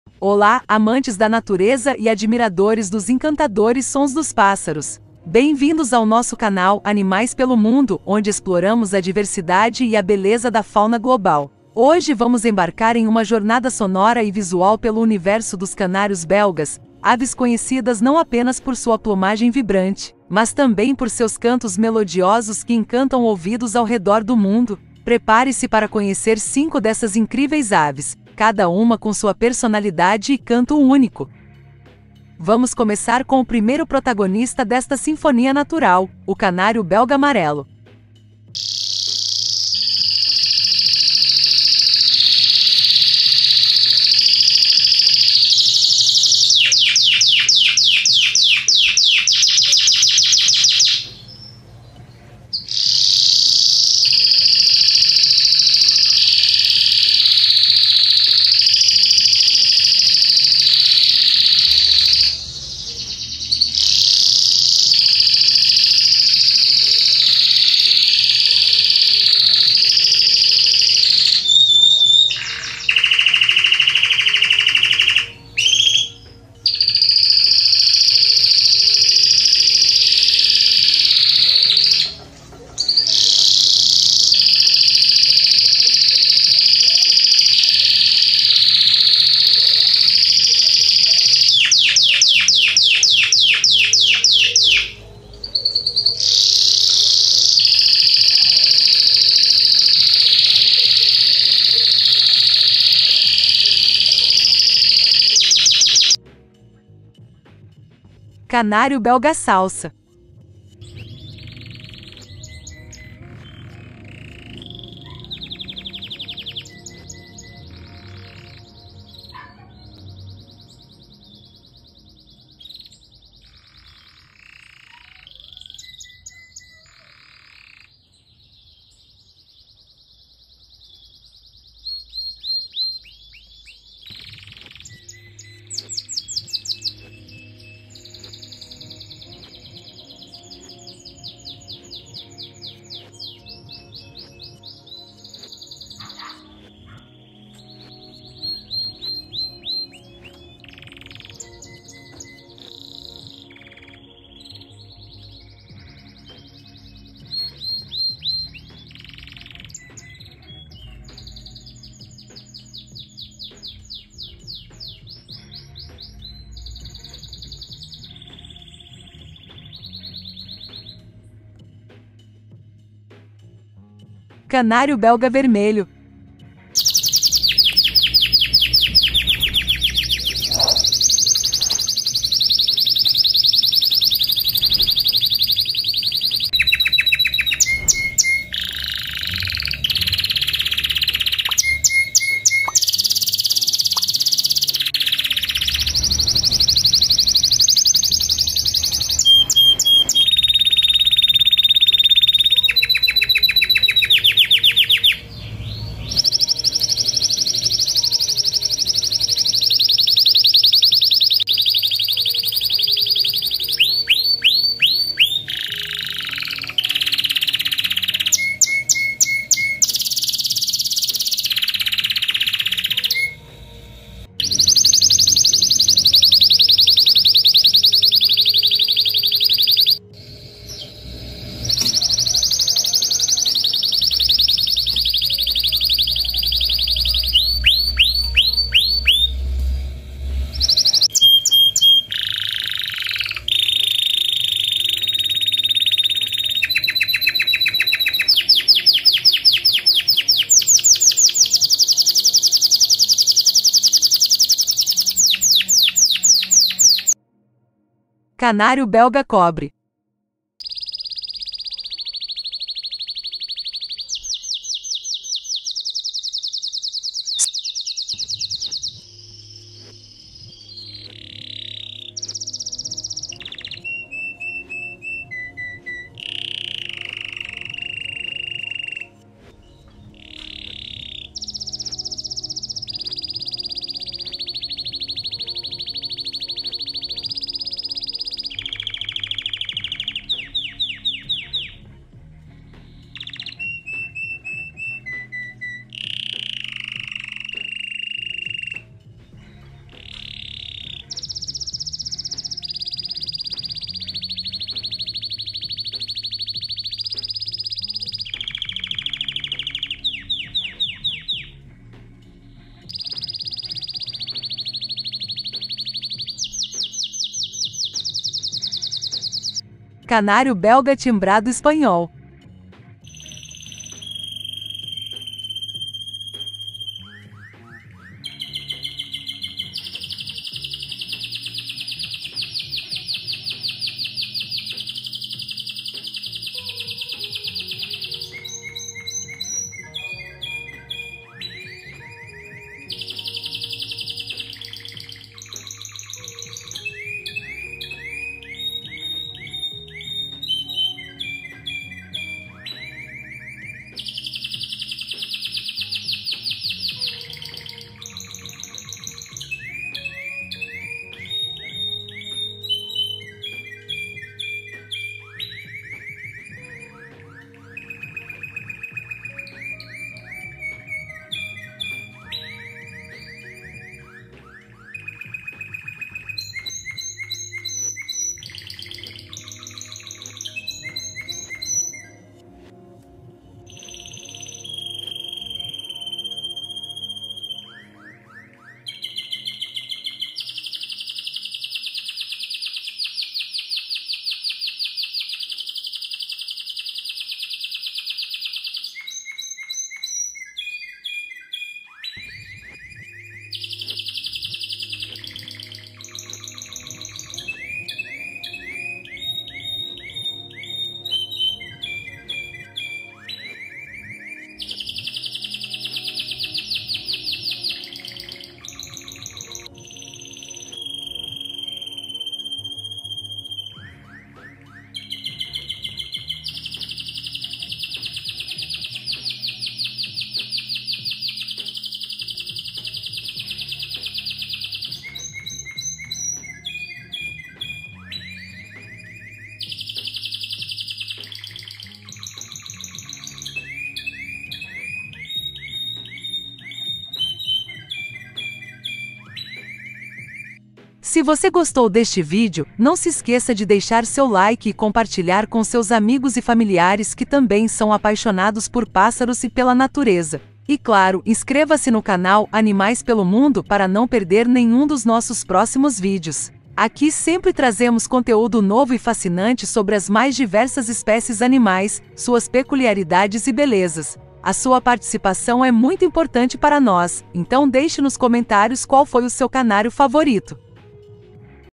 Os Melhores Cantos dos Canários Belgas: Top 5
Descubra os encantadores cantos dos Canários Belgas neste vídeo especial do canal "Animais pelo Mundo". Apresentamos cinco dessas aves incríveis, cada uma com seu próprio estilo e melodia. Conheça o Canário Belga Clássico, o Roller, o Timbrado, o Malinois e o Harz, e deixe-se envolver pela sinfonia natural que eles criam. Se você é apaixonado por pássaros e ama a natureza, este vídeo é perfeito para você! Aprecie a beleza e a música desses pequenos artistas da natureza!